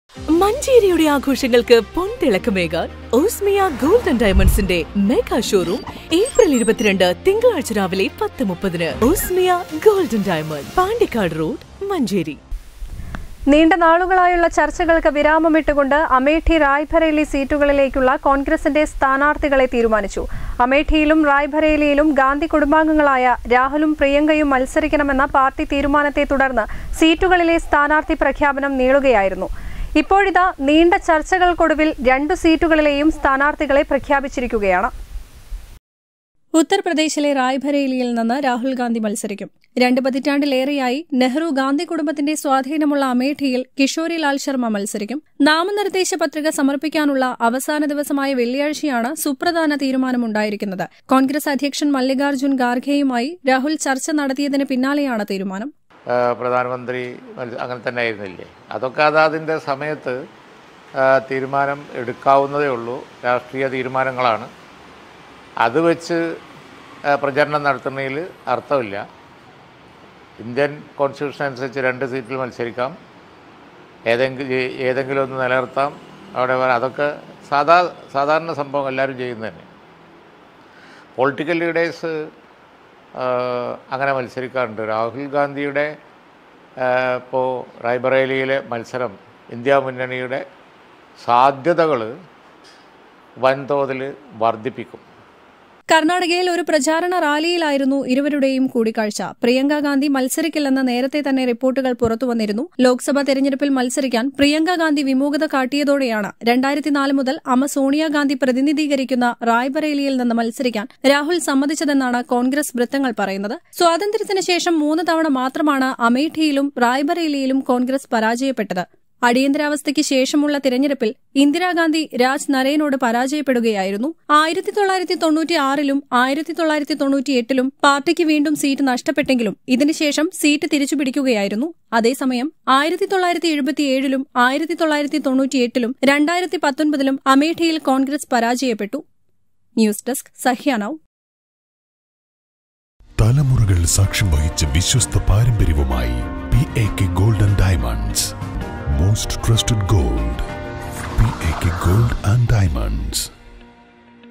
റായ്ബറേലിയിൽ രാഹുൽ ഗാന്ധി മത്സരിക്കും |SAHYA NOW
മല്ലികാർജുൻ ഖാർഗെയുമായി രാഹുൽ ചർച്ച നടത്തിയതിന് പിന്നാലെയാണ് തീരുമാനം.